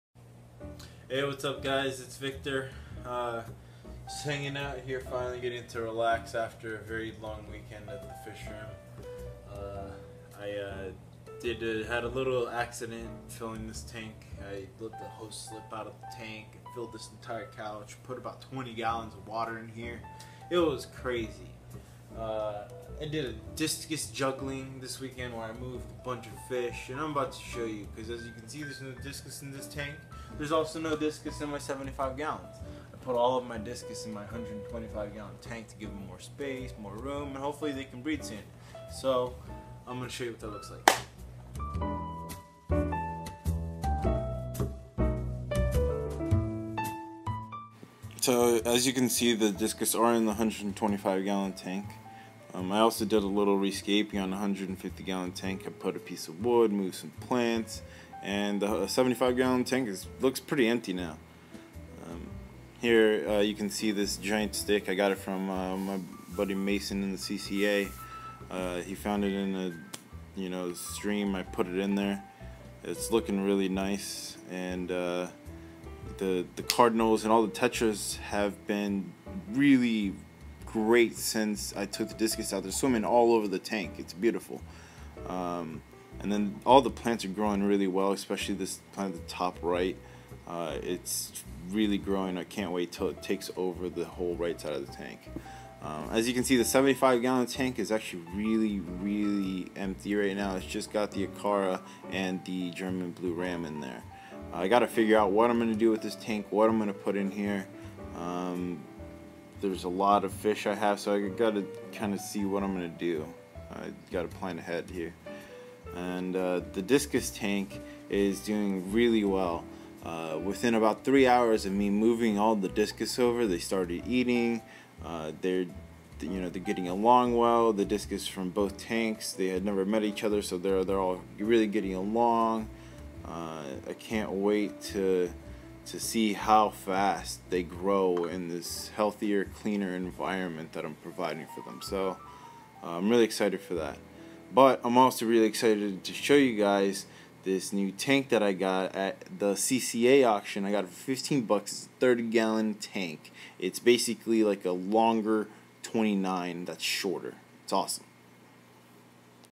The New Discus Tank
This weekend I consolidated all of my discus into the 125G bare bottom tank. I took the ones from the 150G and 75G and put them in there so they'd be in a healthier environment. They all already seem to be doing a lot better than when they were in the other tanks.